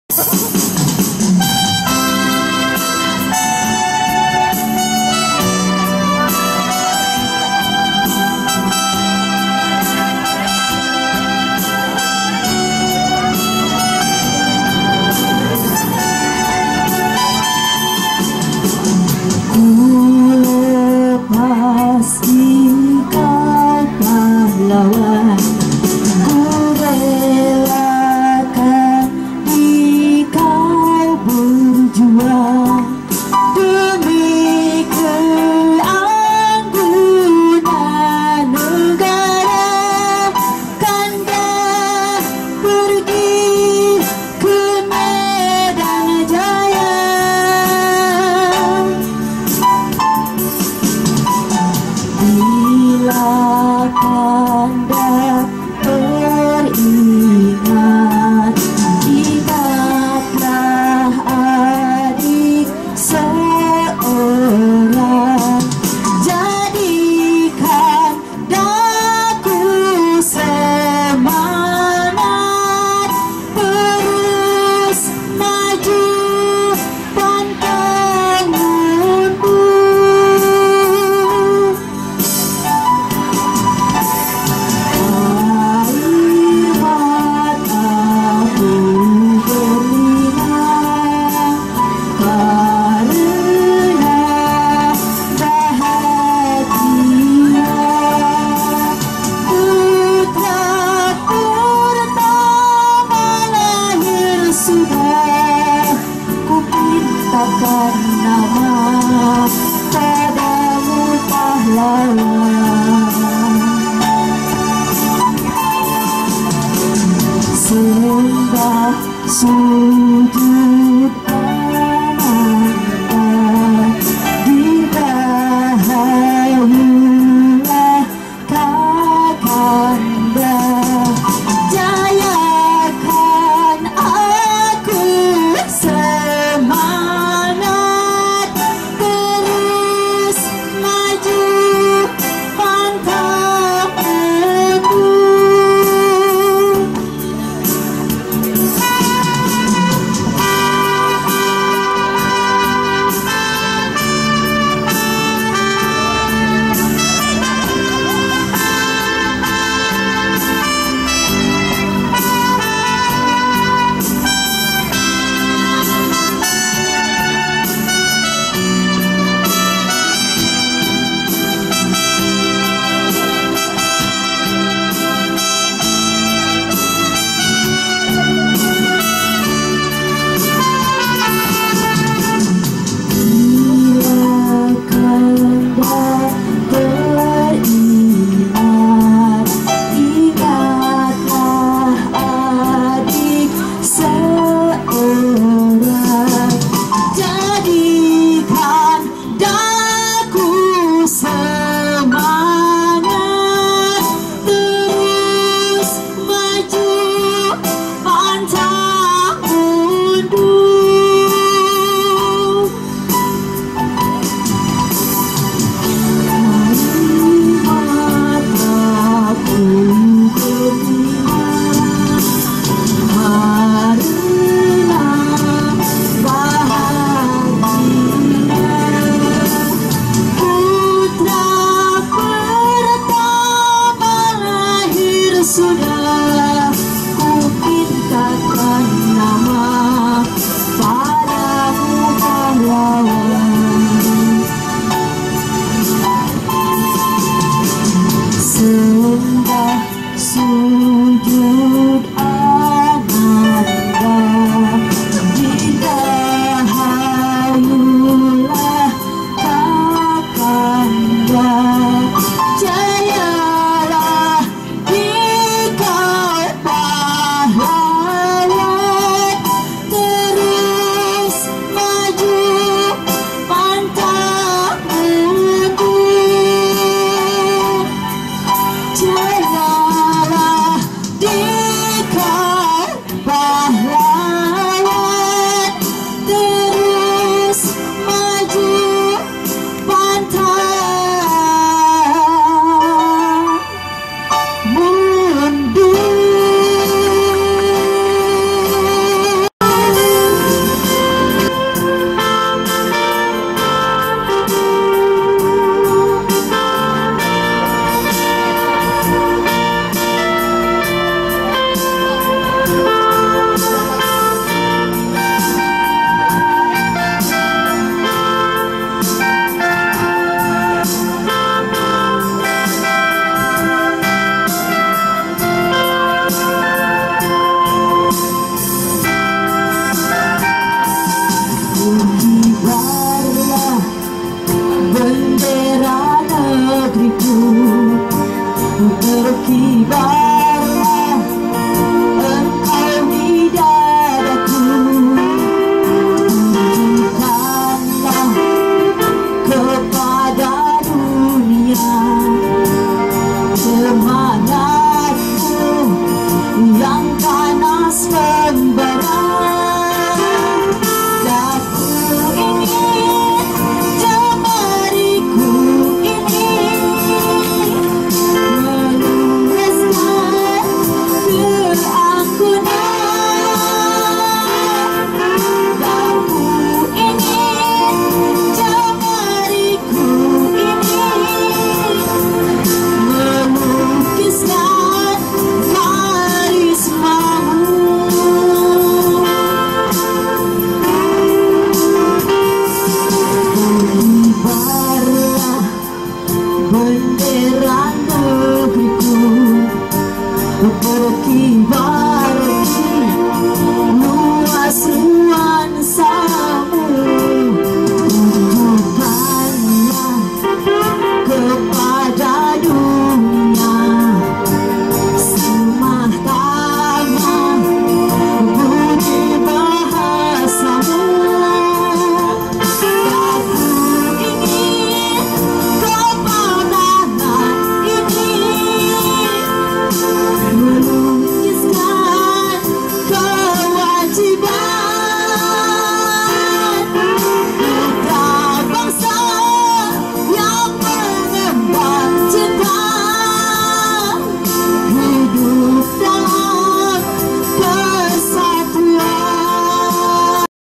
Gebyar Lagu Perjuangan 7); HUT RI 71 Kecamatan Haurwangi
Memperingati HUT RI ke 71, PHBN Kecamatan Haurwangi menyelenggarakan Lomba Menyanyi Lagu Perjuangan